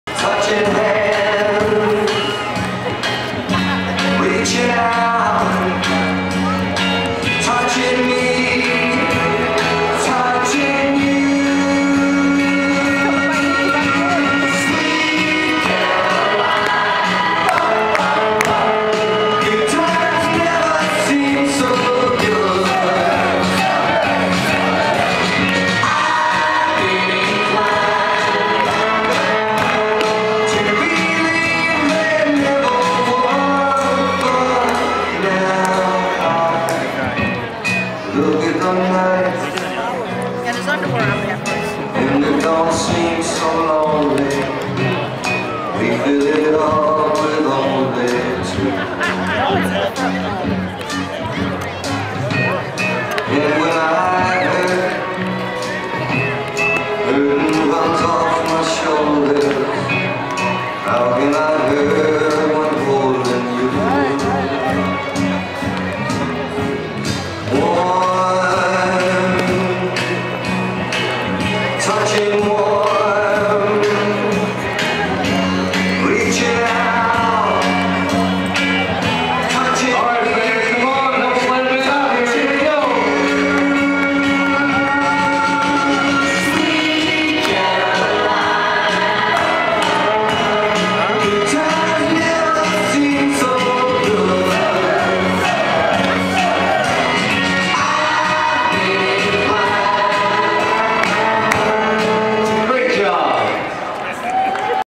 Slam Man
Slam Man singing to the crowd at Silver Cross field.